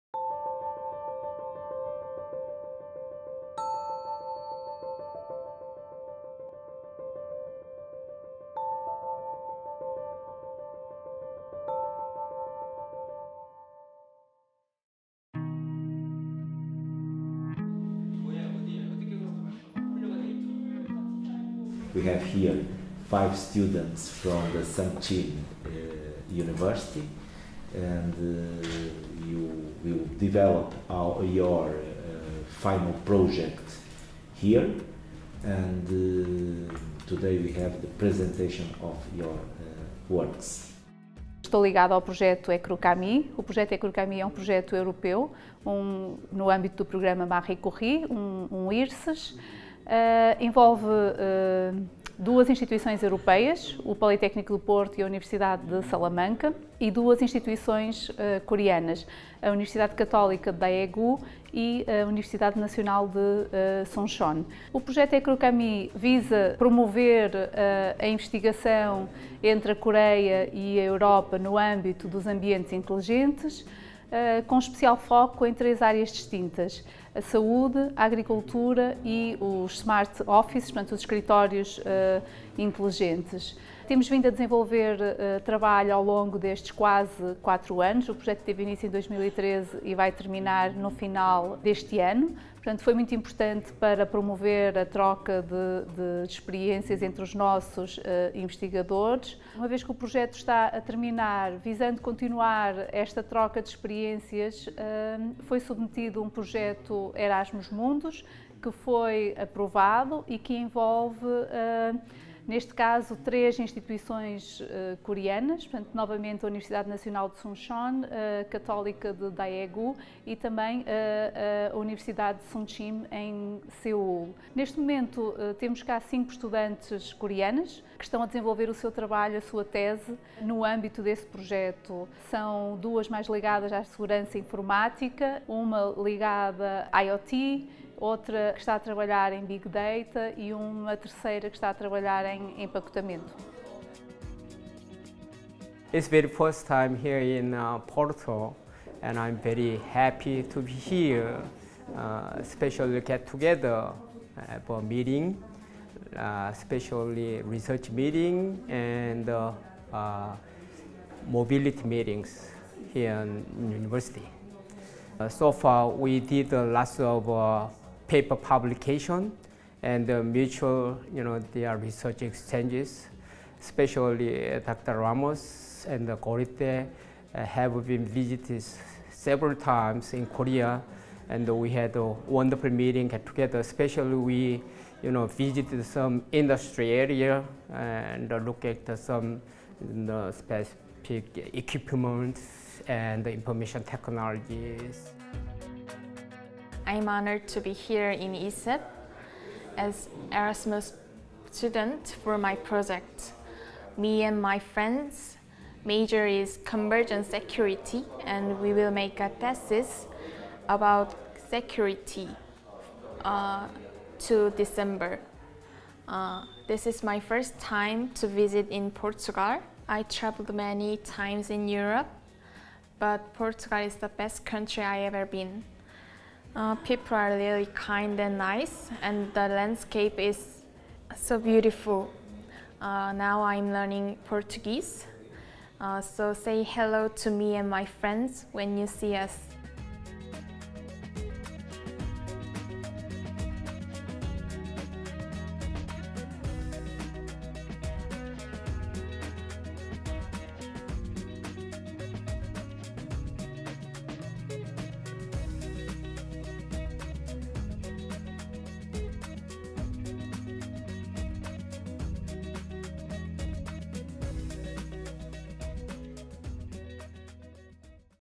P.PORTO TV | Apresentação de projetos de estudantes sul-coreanos em mobilidade
Carlos Ramos, Vice-Presidente do P.PORTO, liderou, no dia 18 de outubro, terça-feira, o evento de apresentação dos projetos das cinco estudantes que desenvolveram as suas teses graças à parceria entre o Politécnico do Porto e algumas universidades da Coreia do Sul, parceria da qual o projeto EKRUCAmI é o melhor exemplo.